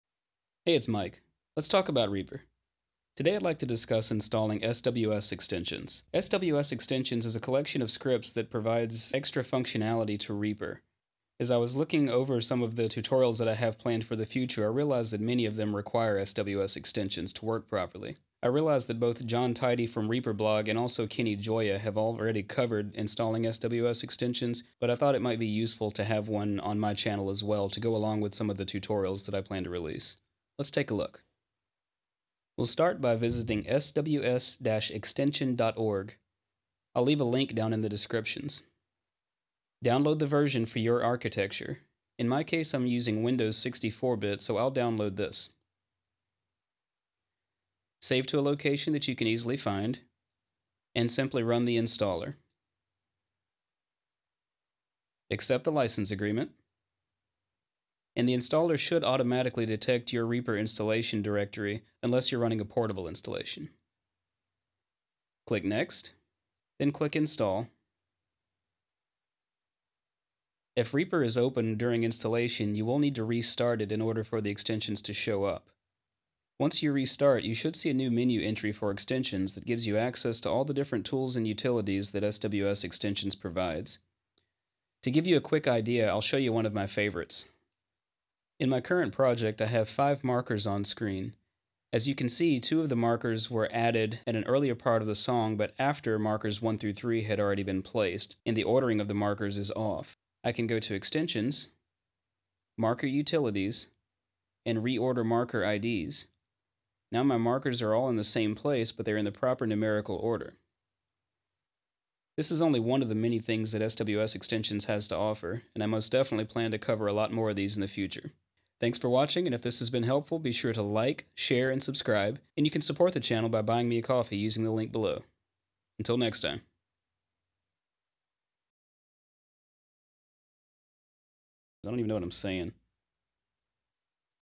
Let's Talk About Reaper - Installing SWS Extensions
Today we'll be looking at installing SWS Extensions to add more functions, features and utilities to Reaper.